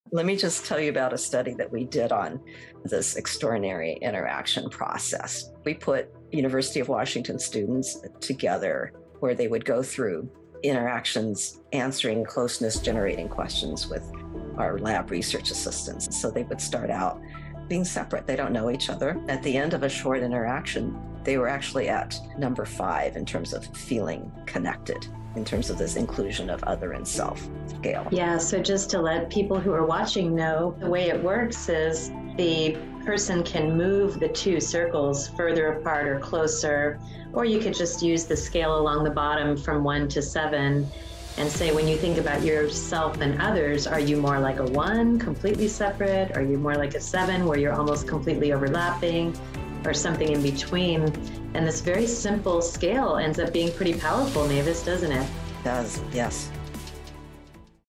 Extraordinary Interactions Study at the University of Washington
In the talk, "How to Decrease Loneliness and Make New Meaningful Friendships at Any Age," Dr. Mavis Tsai, founder of the ACL Global Project, and Dr. Cassi Vieten delve into the intricacies of a University of Washington study on extraordinary interactions. Their discourse underscores the profound implications of human connection within a research context. The talk was recorded as part of the Mental Health & Well-Being Global Summit.

00:00 UW Study
00:15 Extraordinary Interactions Scale
00:43.4 More like a one?
00:45.9 More like a seven?
00:56 Marker